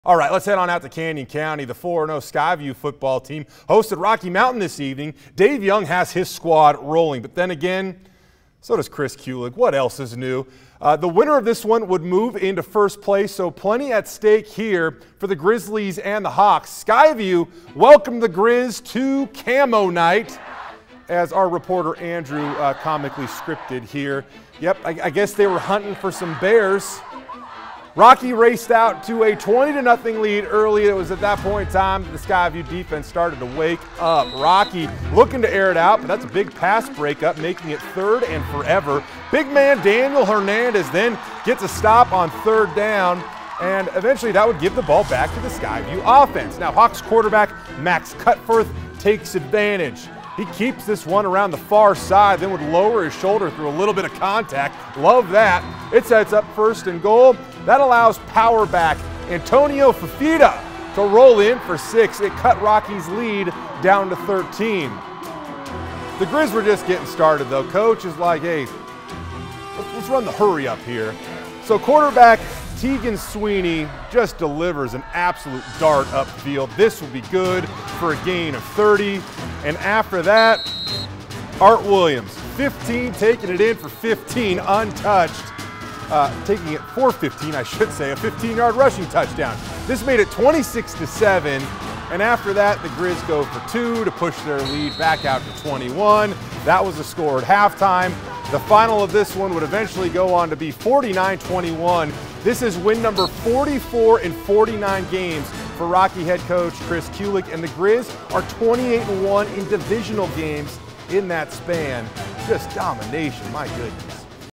Friday Night Football: Skyview and Rocky Mountain fight for the top spot in the 5A SIC
The 3-1 Grizz took on the 4-0 Hawks on Friday night to determine who sits atop the 5A SIC.